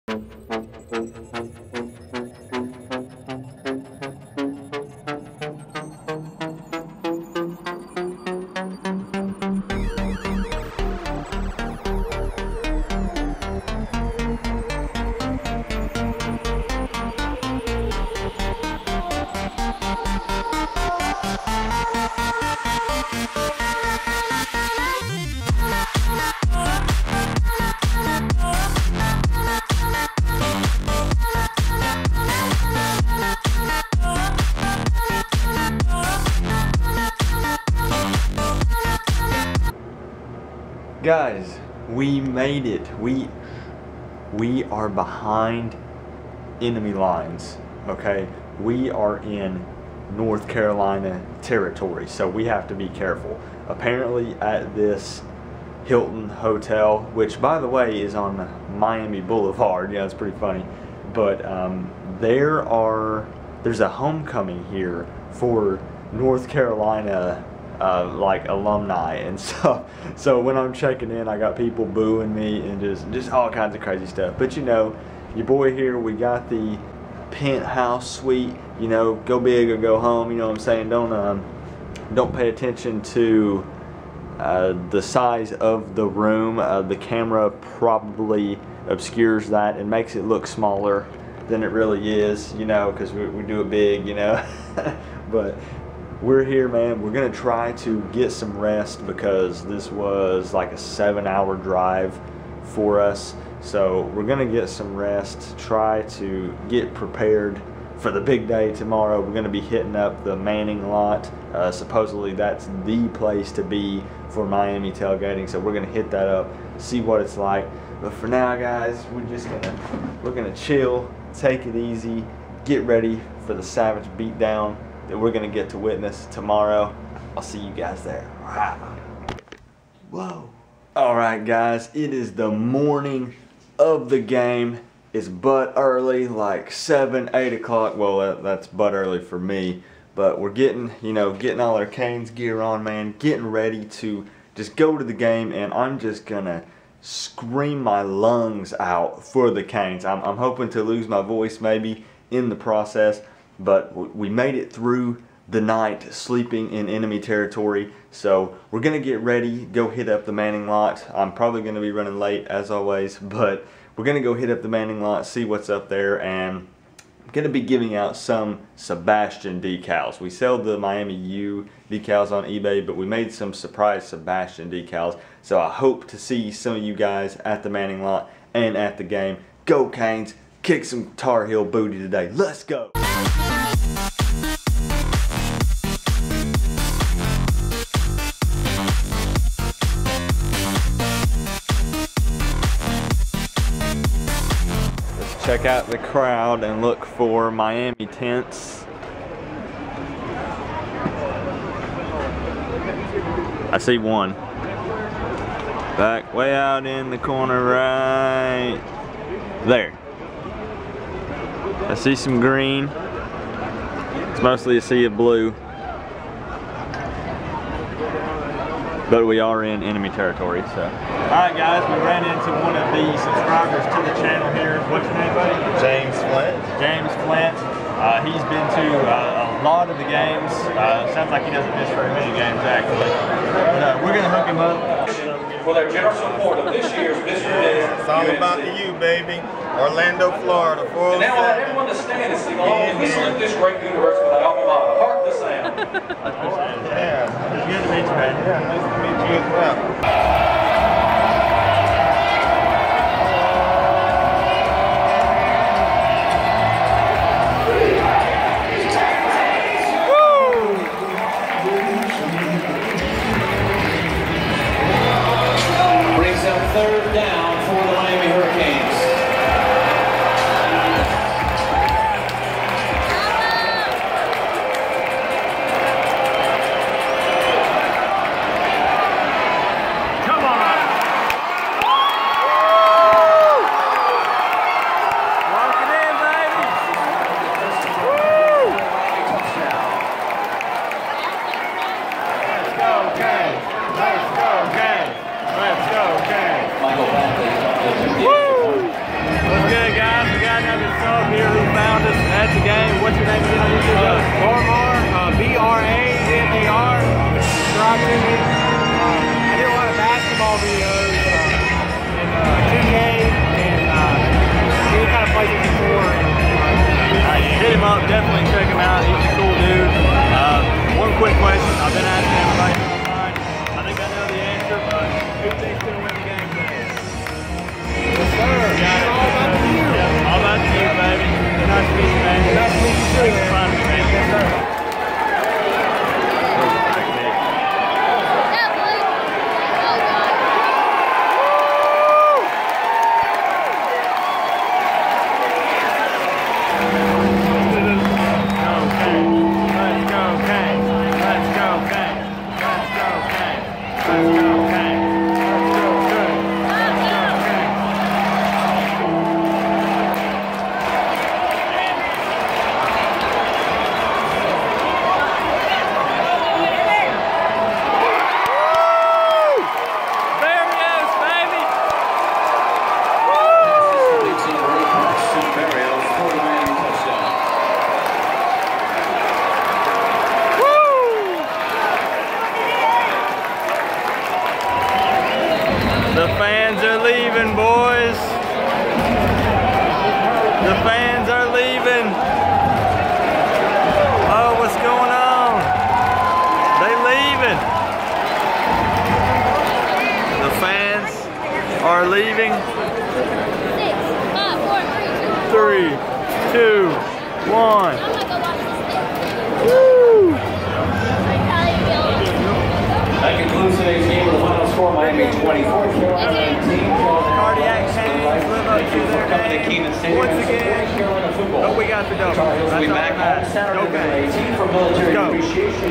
Sights and Sounds Miami Hurricanes vs UNC 2017
WE WERE AT THE GAME! Met a lot of great Miami fans!

My wife makes Miami decals! Show your LOVE for THE U: